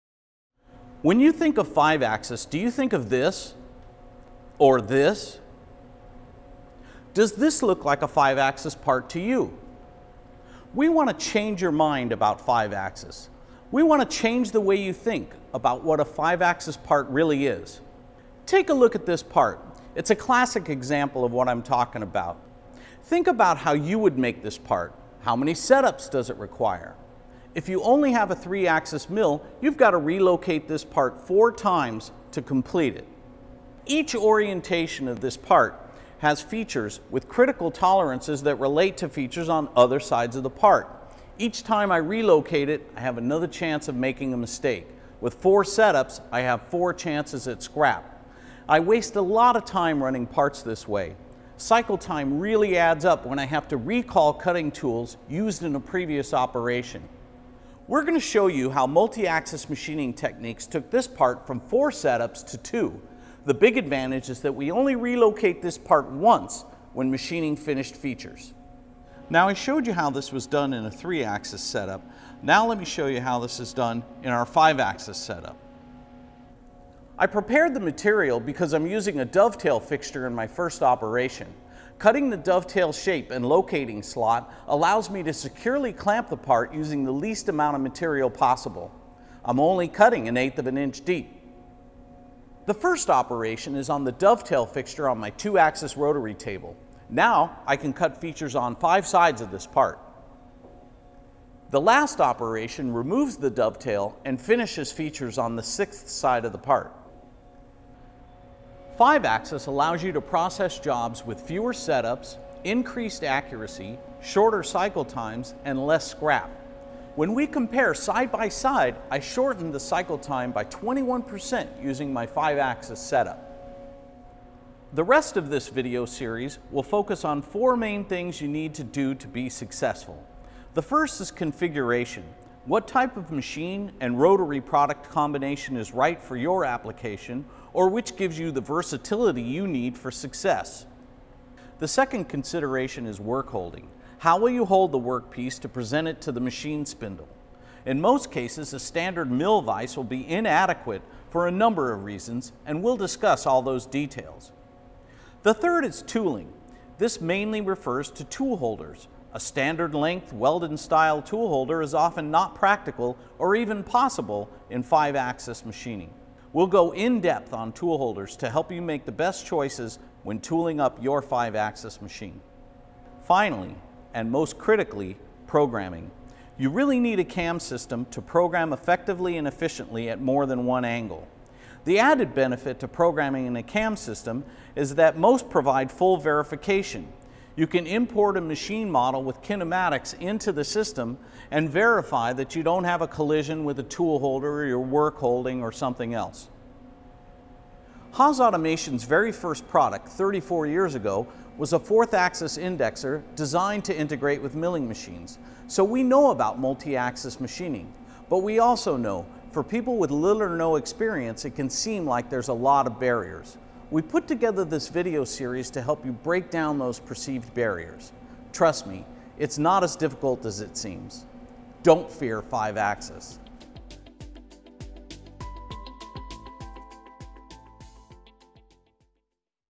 Don't Fear 5-Axis - Episode 1
In the first video in our new series, Don't Fear 5-Axis, Sr. Applications Engineer John Nelson explains why 5-axis machining is much simpler than it has ever been before, and why any shop can be a 5-axis shop.